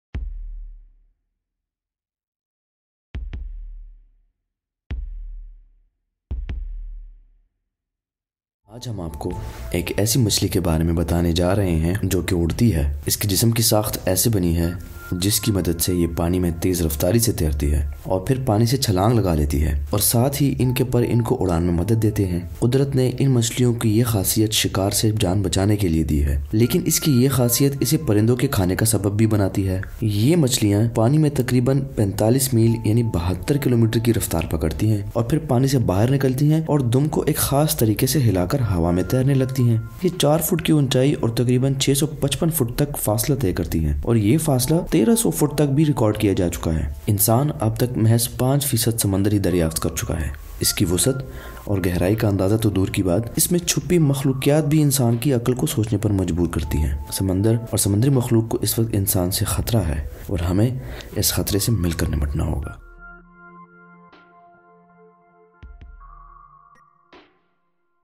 WO MACHLI JO HAWA MA URTI HA | A Fish that can FLY | Urdu| Hindi
Flying Fish - Wo machli jo hawa ma urti ha. In machlio ke aik khasiat ye ha k in k per b hain. aye is video ma dekhty ha or janty hain is machli k bary ma.